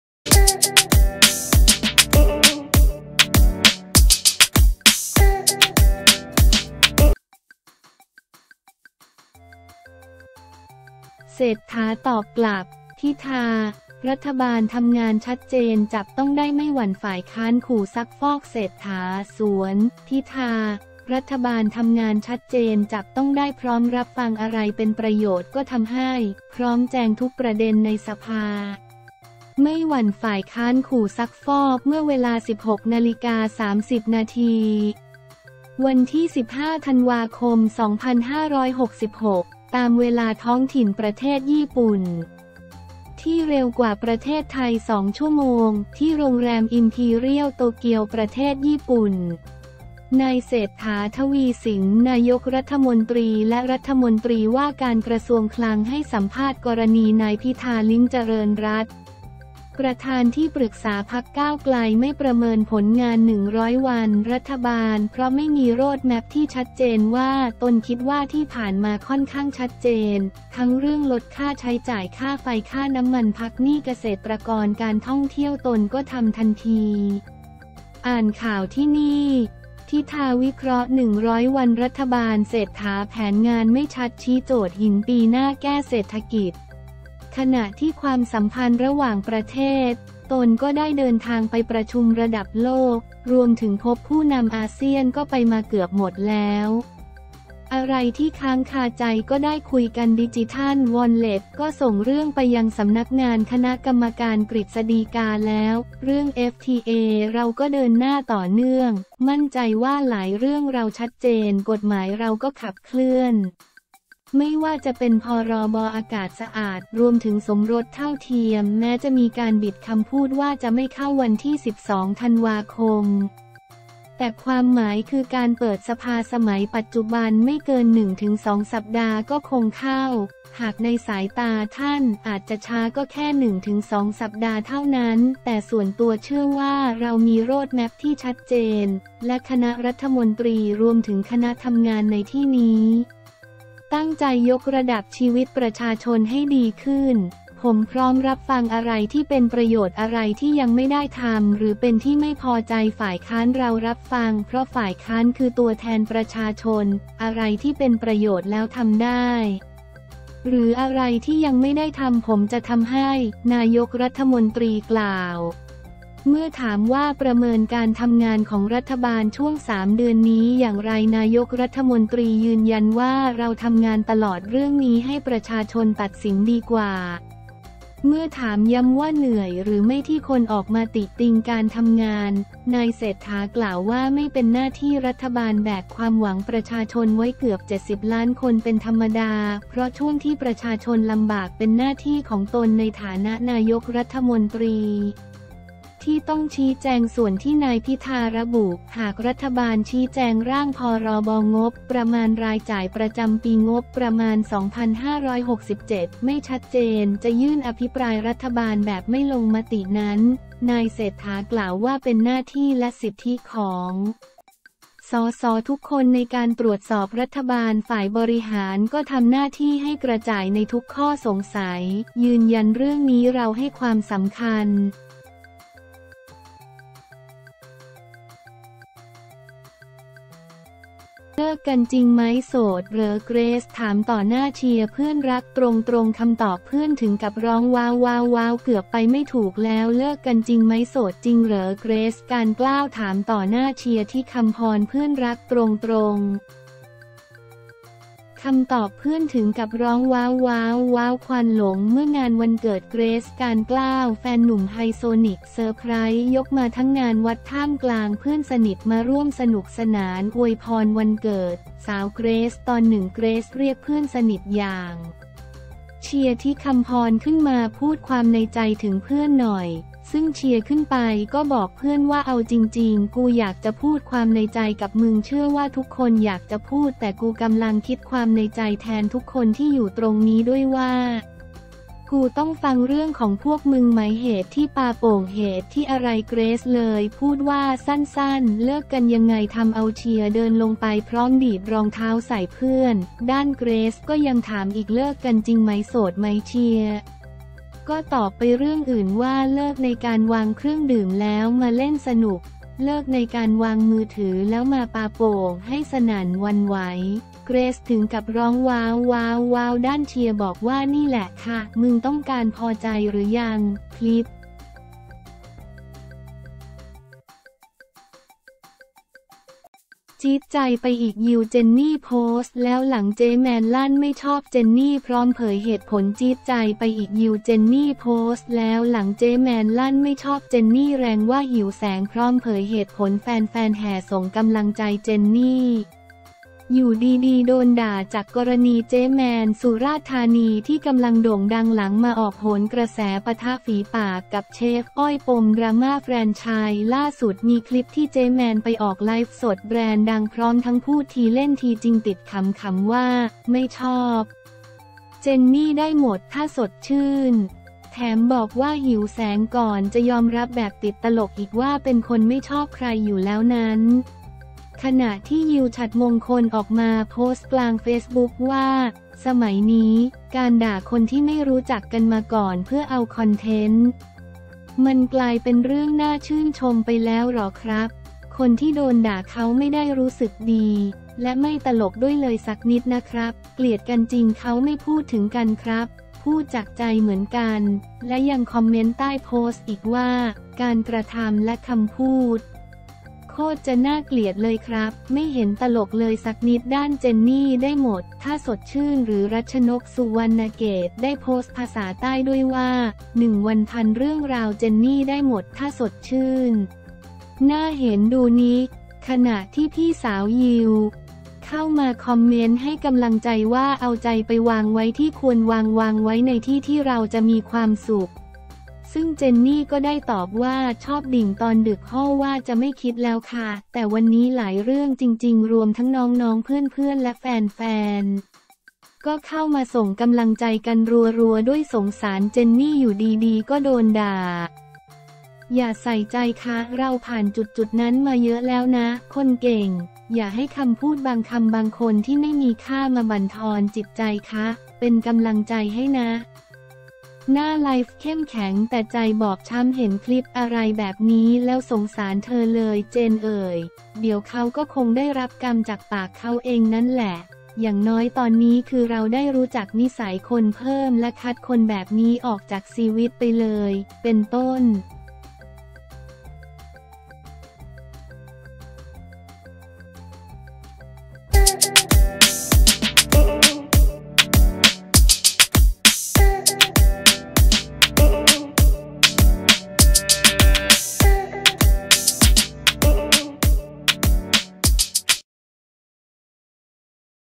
เศรษฐา ตอกกลับ ‘พิธา’ รัฐบาลทำงานชัดเจน จับต้องได้ ไม่หวั่นฝ่ายค้านขู่ซักฟอก
เศรษฐา สวน ‘พิธา’ รัฐบาลทำงานชัดเจน จับต้องได้ พร้อมรับฟังอะไรเป็นประโยชน์ก็ทำให้ พร้อมแจงทุกประเด็นในสภา ไม่หวั่นฝ่ายค้านขู่ซักฟอก

เมื่อเวลา 16.30 น. วันที่ 15 ธ.ค.2566 (ตามเวลาท้องถิ่นประเทศญี่ปุ่น ที่เร็วกว่าประเทศไทย 2 ชั่วโมง) ที่โรงแรมอิมพีเรียลโตเกียว ประเทศญี่ปุ่น นายเศรษฐา ทวีสิน นายกฯและรมว.คลัง ให้สัมภาษณ์กรณีนายพิธา ลิ้มเจริญรัตน์ ประธานที่ปรึกษาพรรคก้าวไกล ไม่ประเมินผลงาน 100 วันรัฐบาล เพราะไม่มีโรดแมปที่ชัดเจนว่า ตนคิดว่าที่ผ่านมาค่อนข้างชัดเจน ทั้งเรื่องลดค่าใช้จ่าย ค่าไฟ ค่าน้ำมัน พักหนี้เกษตรกร การท่องเที่ยวตนก็ทำทันที
อ่านข่าวที่นี่ : พิธา วิเคราะห์ 100 วัน รัฐบาลเศรษฐา แผนงานไม่ชัด ชี้โจทย์หินปีหน้า แก้เศรษฐกิจ

ขณะที่ความสัมพันธ์ระหว่างประเทศ ตนก็ได้เดินทางไปประชุมระดับโลก รวมถึงพบผู้นำอาเซียนก็ไปมาเกือบหมดแล้ว อะไรที่ค้างคาใจก็ได้คุยกัน ดิจิทัลวอลเล็ตก็ส่งเรื่องไปยังสำนักงานคณะกรรมการกฤษฎีกาแล้ว เรื่องเอฟทีเอเราก็เดินหน้าต่อเนื่อง

มั่นใจว่าหลายเรื่องเราชัดเจน กฎหมายเราก็ขับเคลื่อน ไม่ว่าจะเป็นพ.ร.บ.อากาศสะอาด รวมถึงสมรสเท่าเทียม แม้จะมีการบิดคำพูดว่าจะไม่เข้าวันที่ 12 ธ.ค. แต่ความหมายคือการเปิดสภาฯ สมัยปัจจุบัน ไม่เกิน 1-2 สัปดาห์ก็คงเข้า
“หากในสายตาท่าน อาจจะช้า ก็แค่ 1-2 สัปดาห์เท่านั้น แต่ส่วนตัวเชื่อว่าเรามีโรดแมปที่ชัดเจน และคณะรัฐมนตรี รวมถึงคณะทำงานในที่นี้ ตั้งใจยกระดับชีวิตประชาชนให้ดีขึ้น ผมพร้อมรับฟัง อะไรที่เป็นประโยชน์ อะไรที่ยังไม่ได้ทำ หรือเป็นที่ไม่พอใจฝ่ายค้าน เรารับฟัง เพราะฝ่ายค้านคือตัวแทนประชาชน อะไรที่เป็นประโยชน์แล้วทำได้ หรืออะไรที่ยังไม่ได้ทำ ผมจะทำให้” นายกฯ กล่าว

เมื่อถามว่าประเมินการทำงานของรัฐบาลช่วง 3 เดือนนี้อย่างไร นายกฯ ยืนยันว่า เราทำงานตลอด เรื่องนี้ให้ประชาชนตัดสินดีกว่า

เมื่อถามย้ำว่าเหนื่อยหรือไม่ที่คนออกมาติติงการทำงาน นายเศรษฐา กล่าวว่า ไม่ เป็นหน้าที่รัฐบาล แบกความหวังประชาชนไว้เกือบ 70 ล้านคนเป็นธรรมดา เพราะช่วงที่ประชาชนลำบากเป็นหน้าที่ของตนในฐานะนายกฯ ที่ต้องชี้แจง

ส่วนที่นายพิธา ระบุหากรัฐบาลชี้แจงร่าง พ.ร.บ.งบประมาณรายจ่ายประจำปีงบประมาณ 2567 ไม่ชัดเจน จะยื่นอภิปรายรัฐบาลแบบไม่ลงมตินั้น นายเศรษฐา กล่าวว่า เป็นหน้าที่และสิทธิของ สส.ทุกคนในการตรวจสอบรัฐบาล ฝ่ายบริหารก็ทำหน้าที่ให้กระจ่ายในทุกข้อสงสัย ยืนยันเรื่องนี้เราให้ความสำคัญ